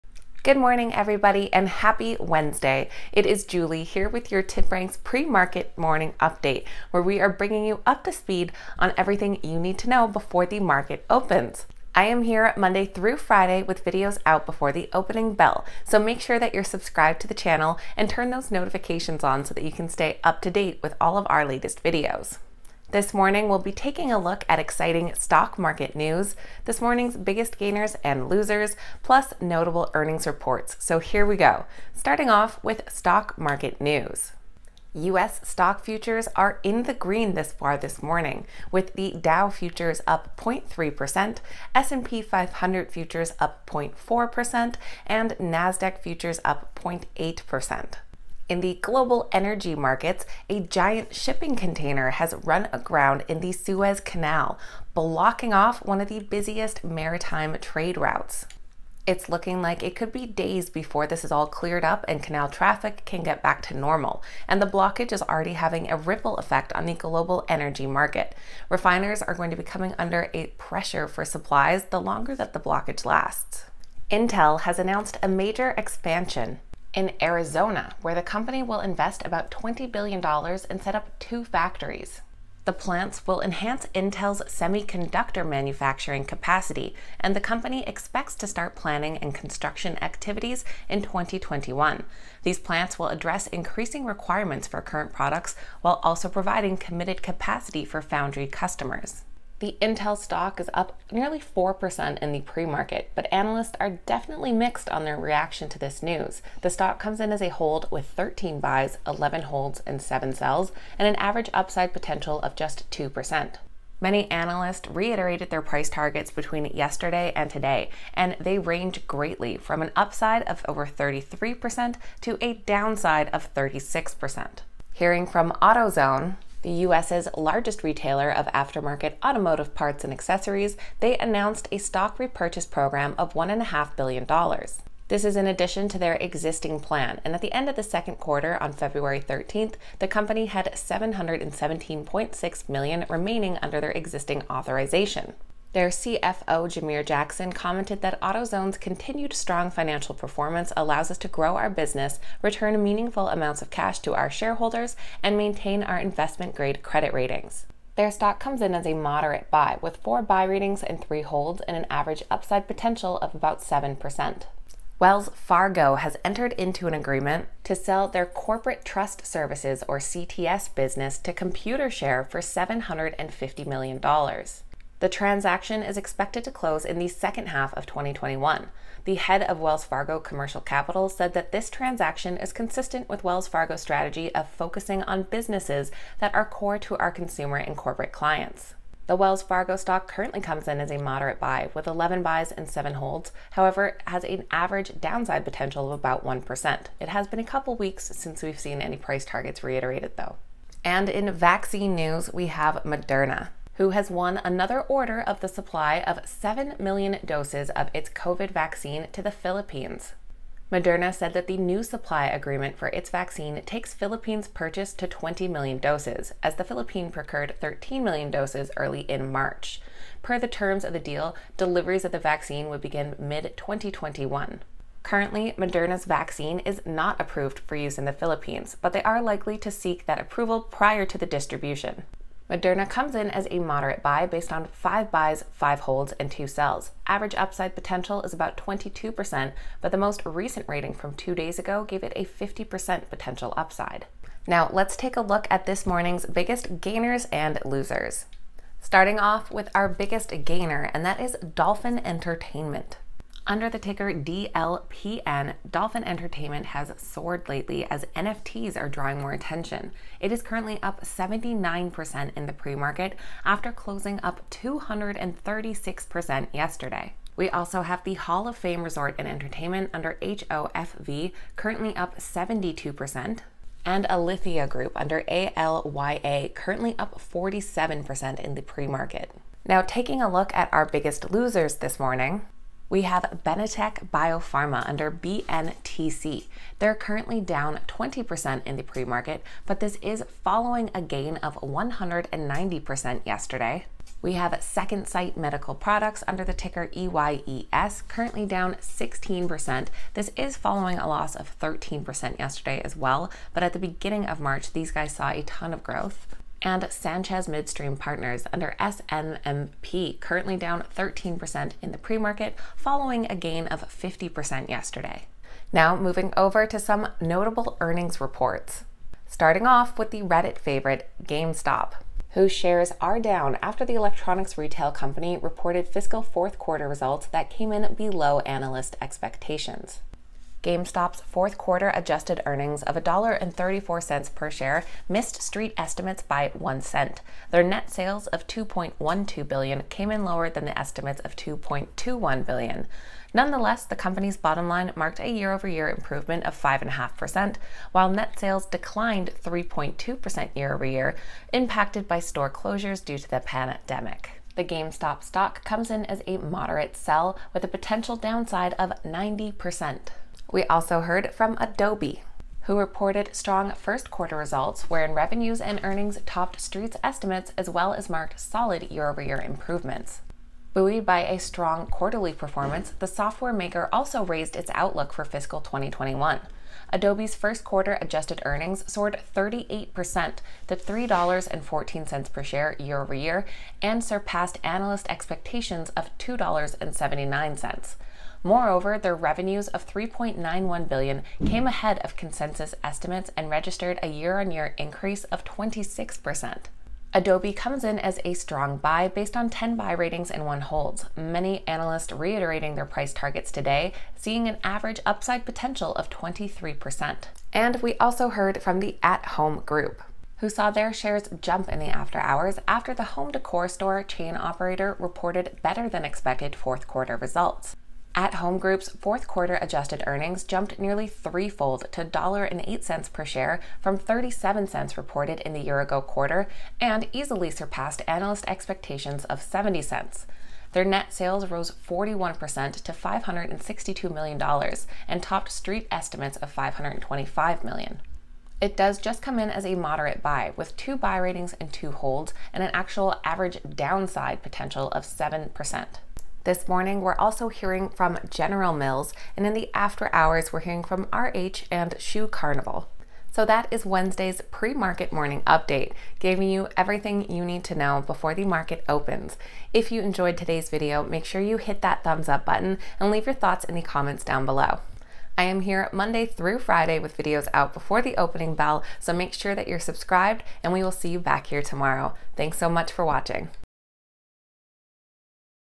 TipRanks Wednesday Pre Market Update! All You Need To Know Before The Market Opens!
TipRanks Pre Market News Today
Daily Market Preview -  Wednesday, March 24, 2021

Watch the TipRanks premarket news and stay up to date before the market opens. We touch on earnings reports, the biggest movers, winners & losers, and big company updates.

Today’s video covers:
1. Stock Market News
2. US stocks on the move
3. Notable Earnings Reports

Stocks:
00:00 Morning Update
00:33 Stock Market News
01:14 Intel (INTC)
02:05 Autozone (AZO)
02:45 Wells Fargo (WFC)
03:22 Moderna (MRNA)
04:11 Dolphin Entertainment (DLPN)
05:21 GameStop (GME)
06:01 Adobe (ADBE)
06:49 At Home Group (HOME)